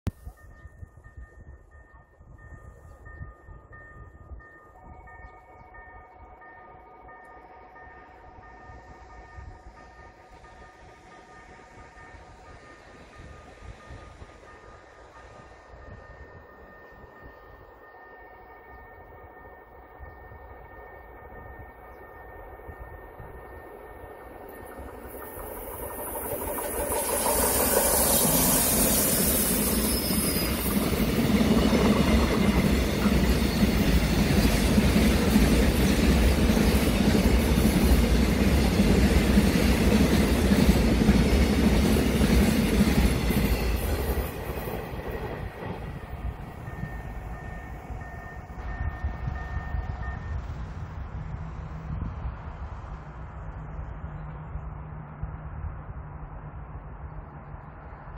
충북선, 시원스러운 시멘트 화물열차 운행음
오봉발 도담행 #3141 정기 화물열차가  충북 음성군 음성역 근처를 지납니다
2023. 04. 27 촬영